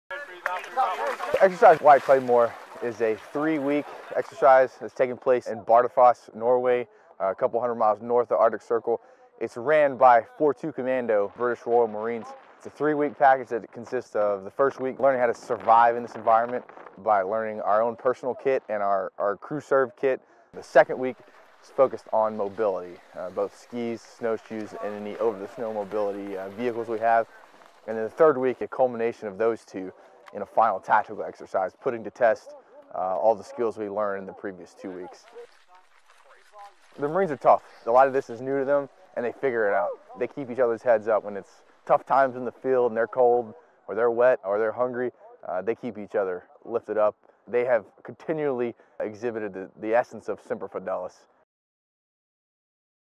White Claymore 2019, Overall 🇺🇸
🔴 [Author]:
Video by Spc. Arrian Matos

🔴 [Unit]:
Defense Media Activity - Europe Africa

🔴 [Location]:
Bardufoss, Norway

🔴 [Description]:
53 sec Overview Story on exercise White Claymore 2019.

00:05 CPT Josh White, Marine Rotational Forces Europe Commanding Officer.

U.S. Marines with Marine Rotational Force-Europe 19.1, Marine Forces Europe and Africa, patrol during Exercise White Claymore in Bardufoss, Norway, Feb. 21, 2019. The training enabled the Marines to improve their over-the-snow movement and combat skills in the arctic climate of northern Norway.

🔴 [Tags]:
Norway, Marines, Overview, MRF-E, White Claymore

🔴 [VIRIN]:
190221-A-GG206-006